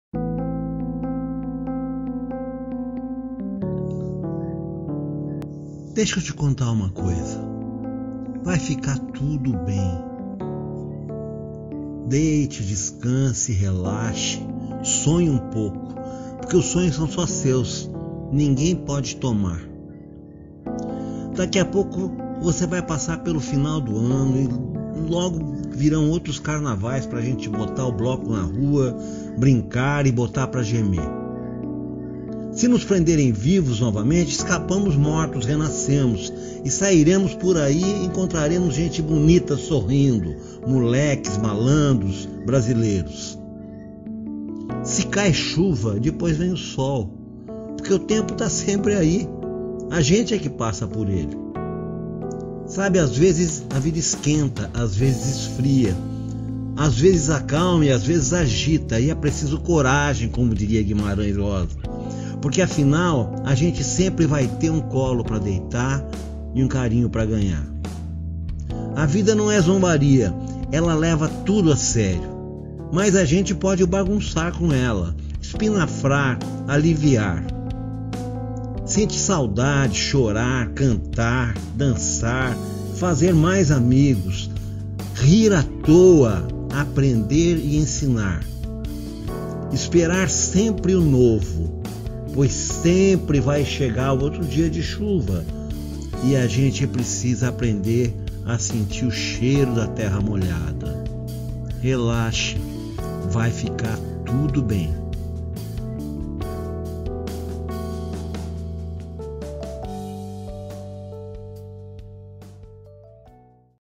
São tempos estranhos, mas, TUDO BEM
Vídeo apresentando  CRÔNICA Vai ficar tudo bem, uma reflexão sobre nossos comportamentos durante a pandemia.